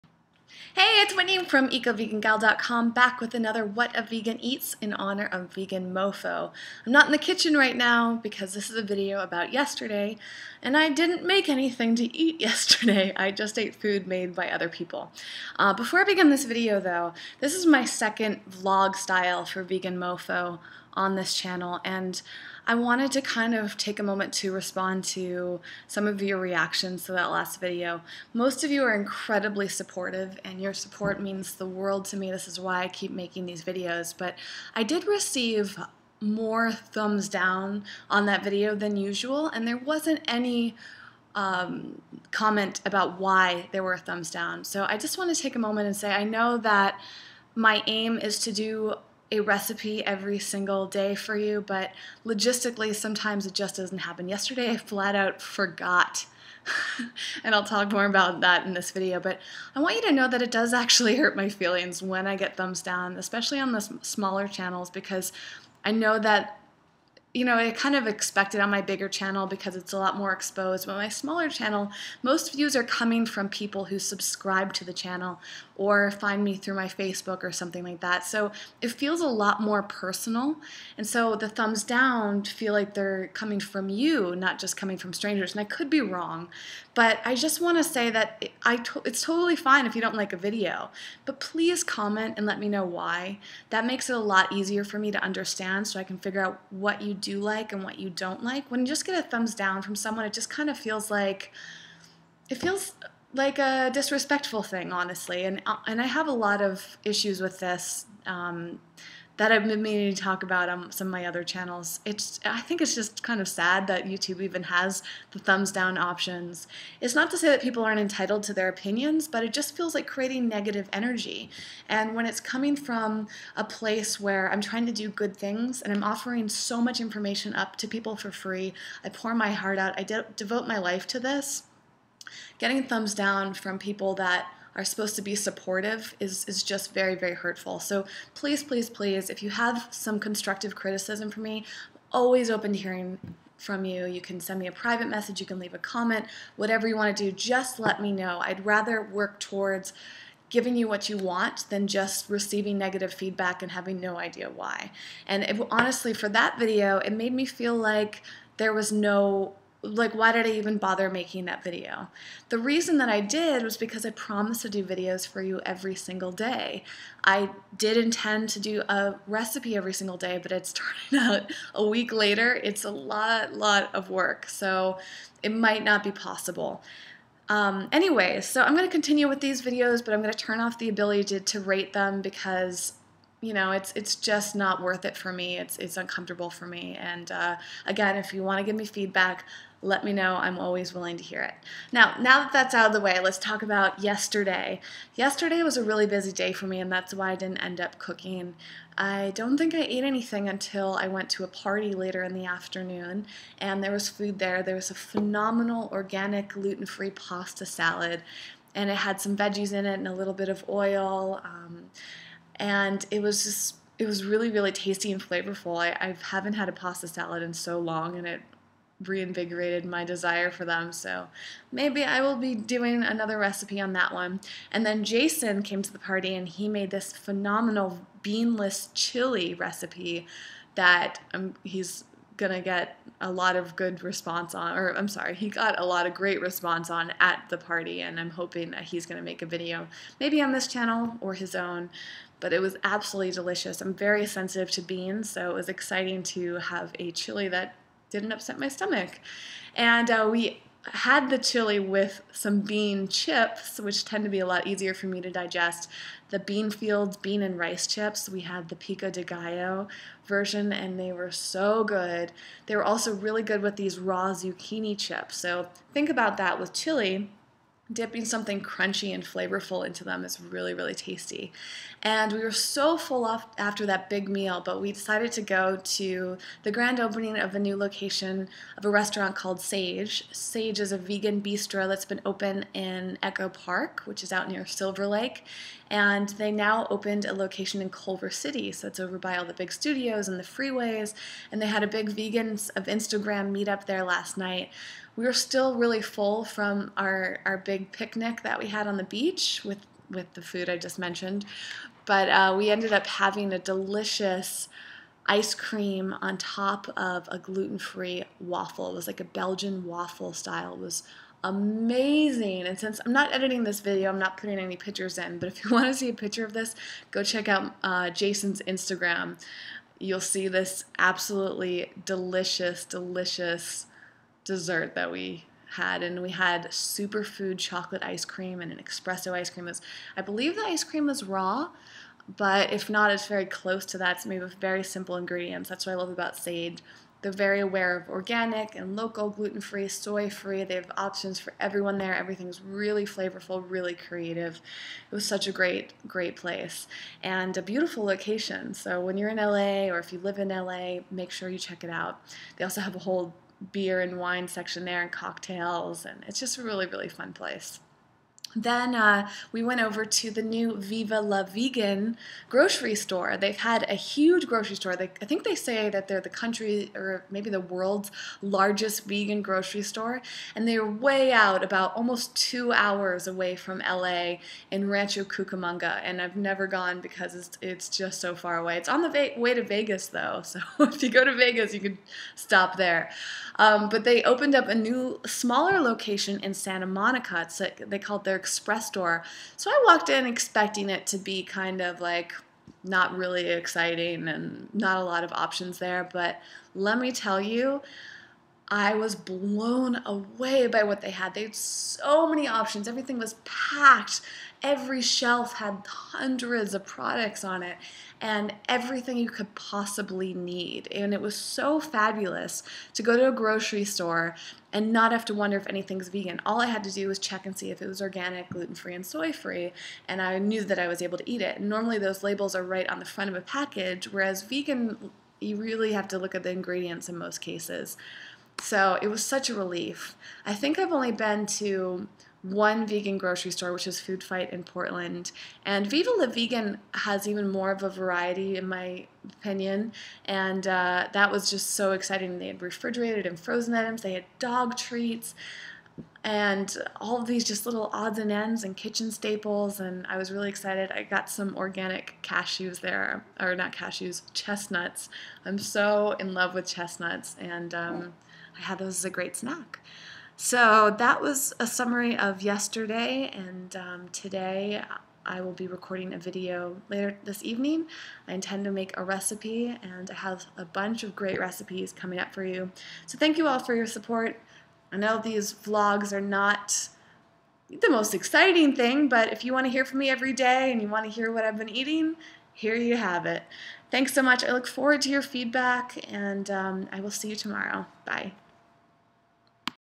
Vegan Mofo Vlog: Picnic, Bistro & Vegan Grocery Store
Whitney chats about foods she ate at a beach picnic, Sage Bistro in Culver City, and her visit to Viva La Vegan in Santa Monica. She also expresses her sadness over some negative video feedback and asks for constructive criticism instead.